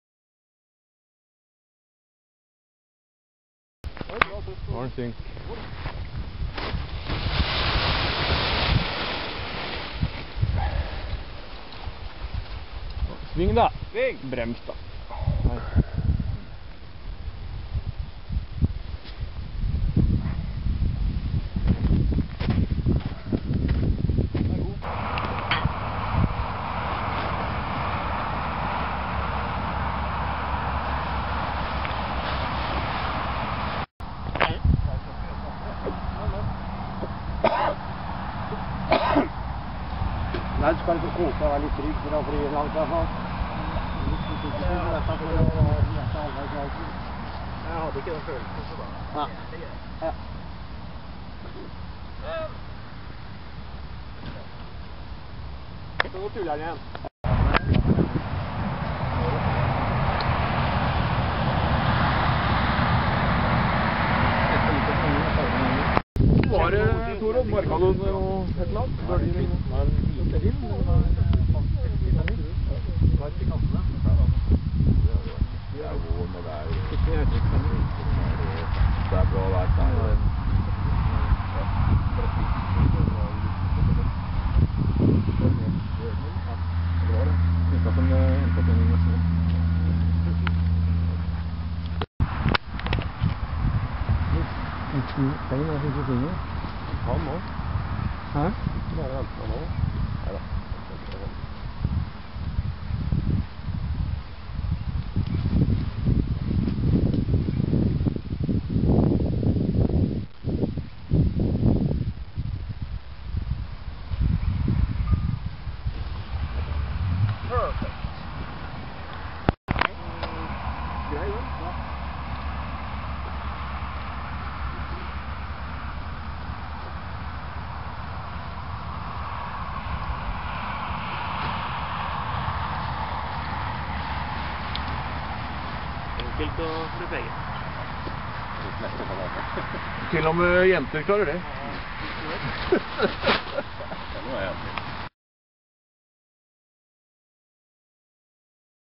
Klubbtur Hof Paragliderklubb
Jeg og Nils Skåden arrangerte klubbtur til Gausdal for Hoff Paragliderklubb i år også.